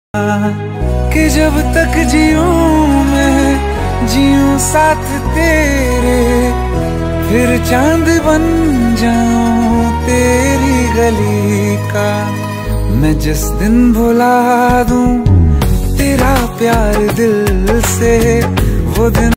"Touching the sky, one floor at a time.
"🌟 Dive into Dubai's Glittering Nightscapes! 🌆 Experience the magic of an enchanting evening drive through the city's vibrant streets. From iconic skyscrapers to mesmerizing reflections, this video is your ticket to a virtual adventure! 🚗🌃 DubaiNights CityLightsMagic ✨"

🧠 - Elevate Your Knowledge Forever